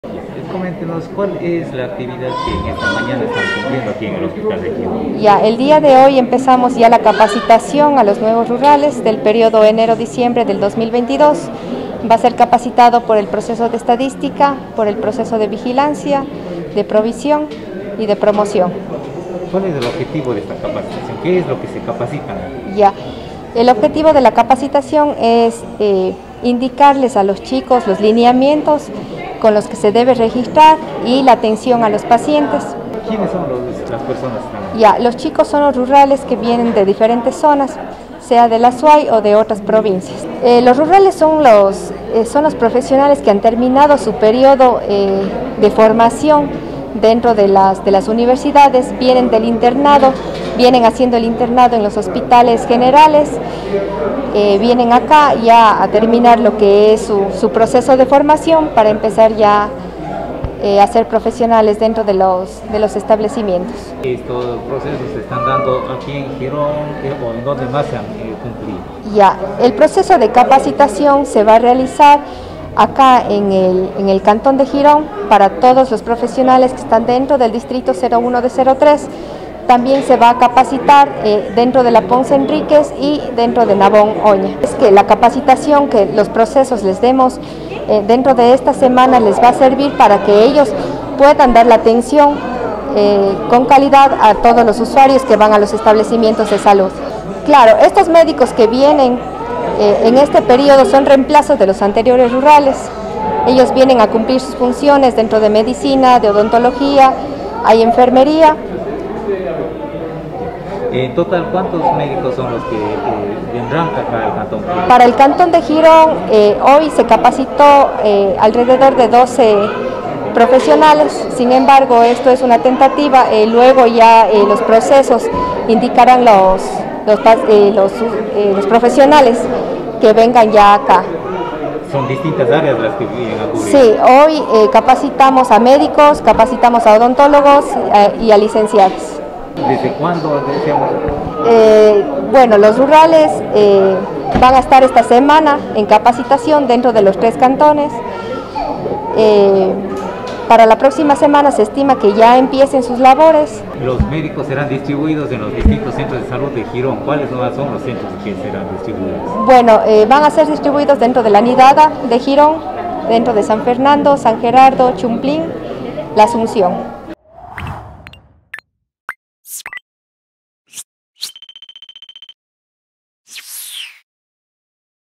Nuevos médicos rurales se capacitan para prestar sus servicios en Girón
En el hospital Aida León de Rodriguez Lara perteneciente al cantón Girón, durante esta semana se desarrolla el proceso de capacitación de los nuevos médicos rurales para el periodo enero-diciembre del año 2022. Un promedio de 12 profesionales en medicina, odontología y enfermería cubrirán los espacios que dejarán quienes ya cumplieron su proceso de formación.